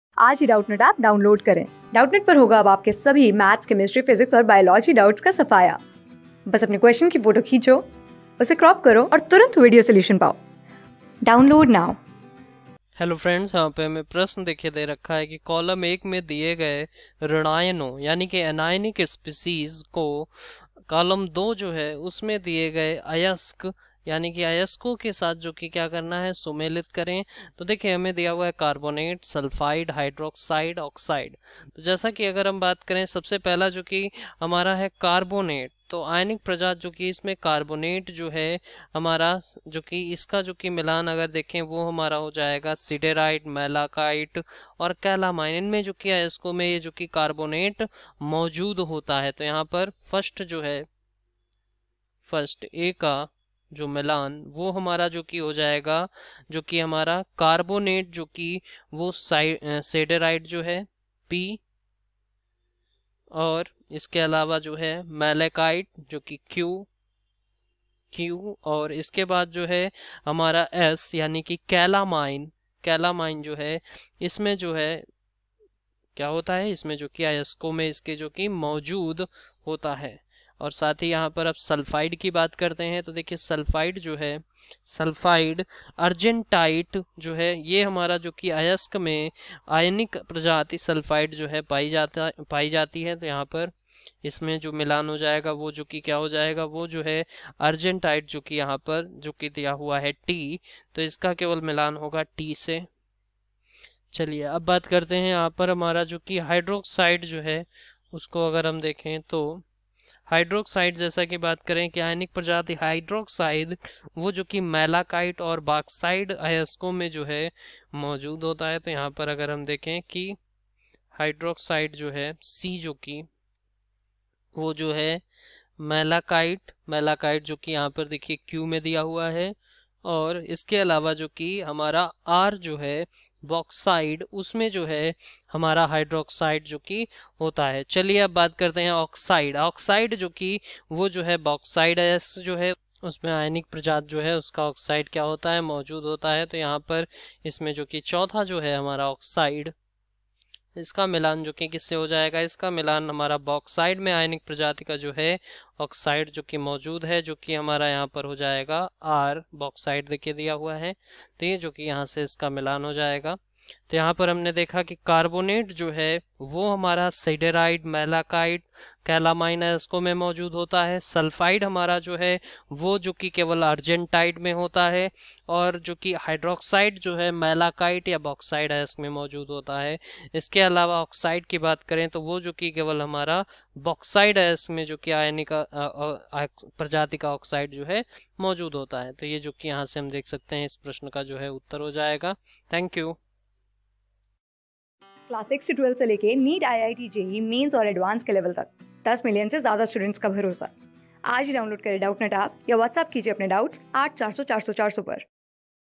कॉलम – I में दिये गये ऋणायनों (anionic species) को कॉलम – II में दिये गये सही अयस्क (अयस्कों) के...
कॉलम – I में दिये गये ऋणायनों (anionic species) को कॉलम – II में दिये गये सही अयस्क (अयस्कों) के साथ सुमेलित करें।
Class: 12
Subject: CHEMISTRY
Chapter: JEE ADVANCED पेपर - 1 (2015)
Board:IIT JEE

👉 Have Any Query? Ask Us.